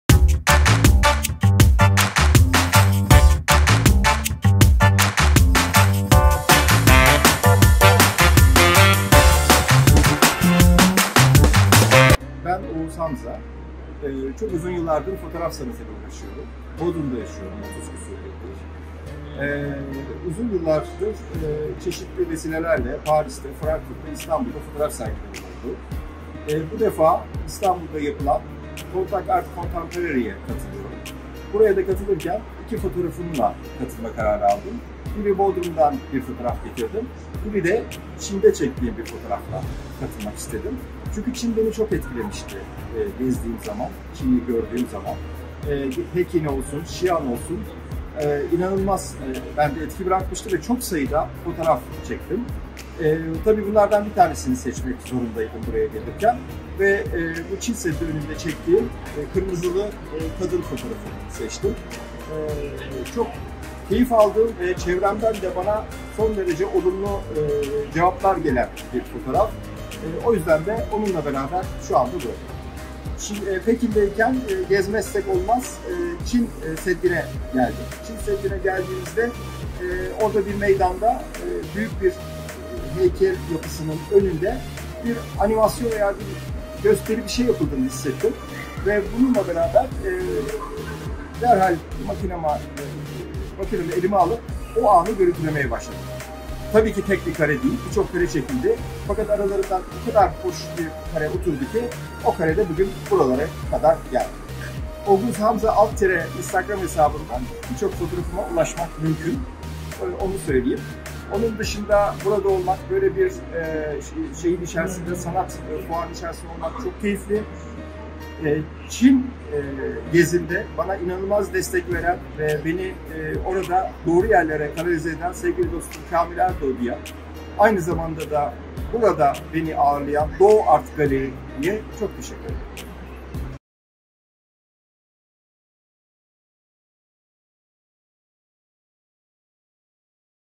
Artcontact İstanbul 2. Çağdaş Sanat Fuarı kapılarını açtı
Oğuz Hamza sergiye Beijing ve Bodrum’da ölümsüzleştirdiği iki fotoğraf ile katıldı